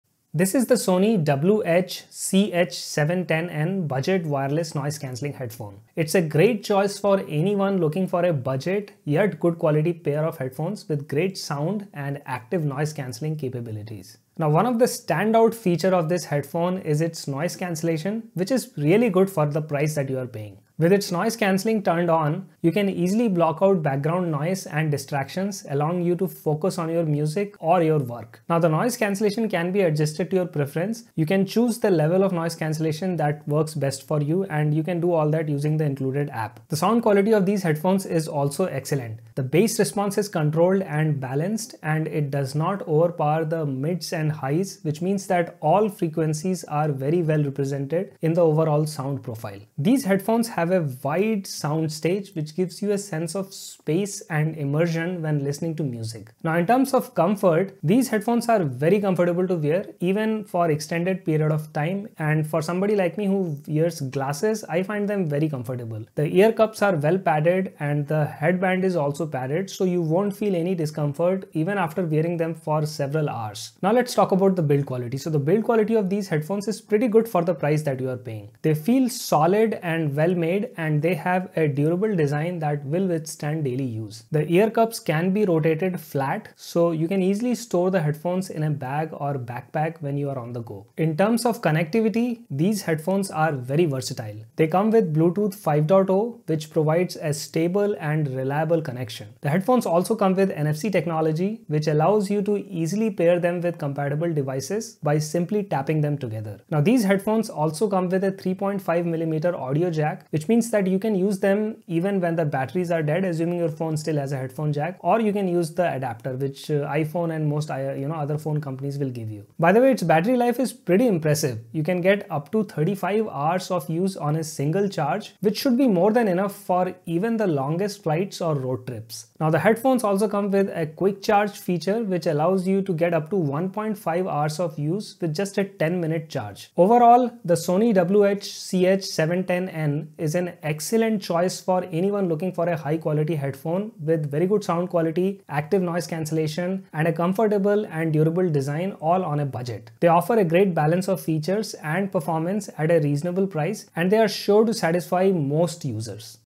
Sony WH-CH710N Wireless ANC Headphones 2023: Are They Any Good?
Should you buy Sony WH-CH710N wireless noise canceling headphones in 2023?

🙏 If you found this video useful, please consider supporting this channel